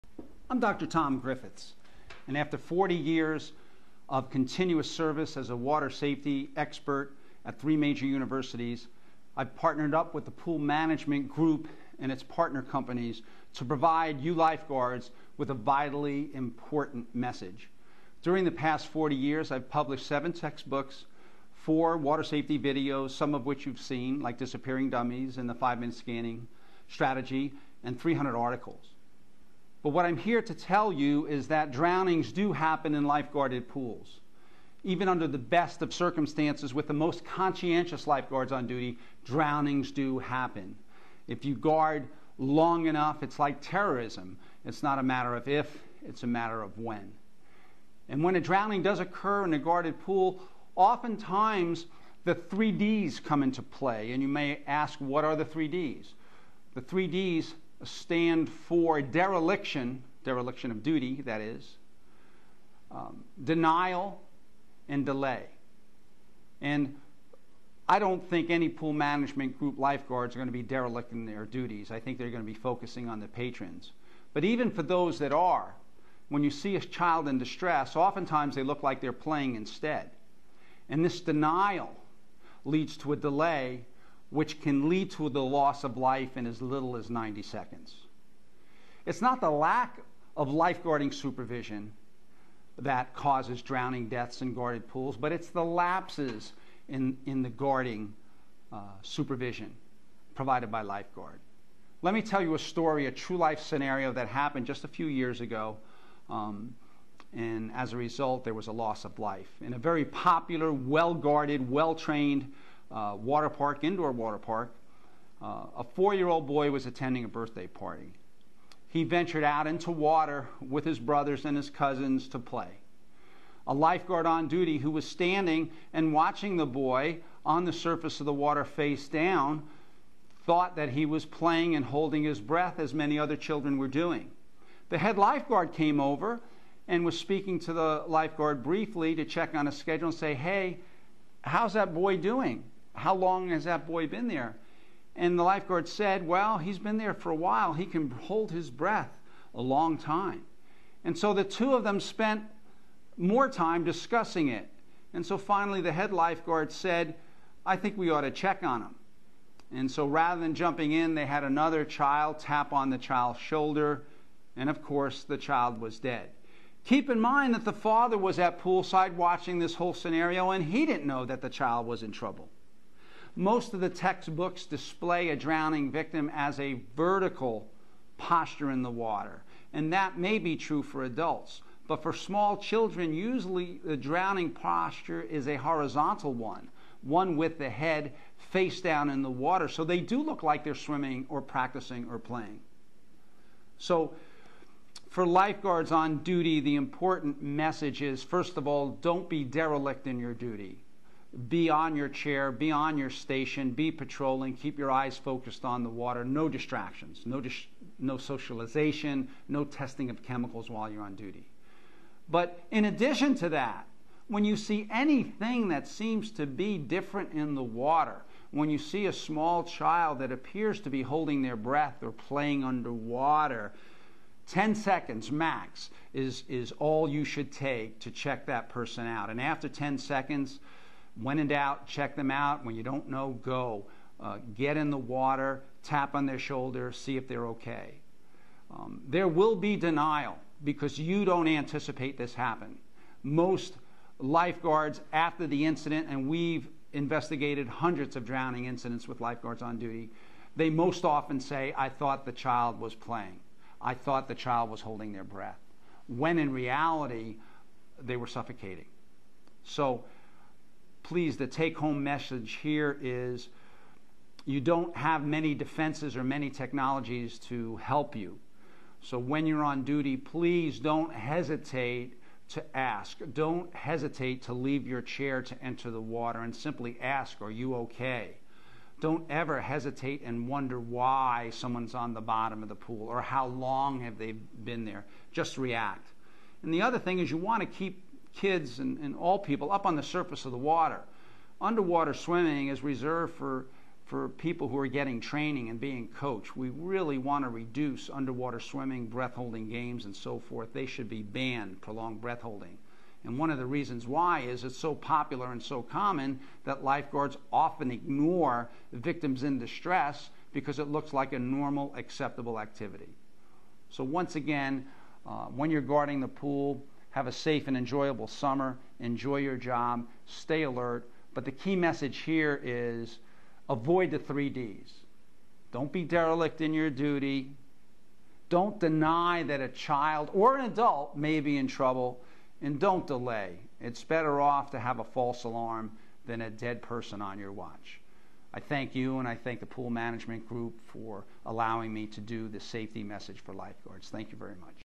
Lifeguard 3D's
Lifeguard training - Dr. Tom Griffiths describes the 3D's for Lifeguards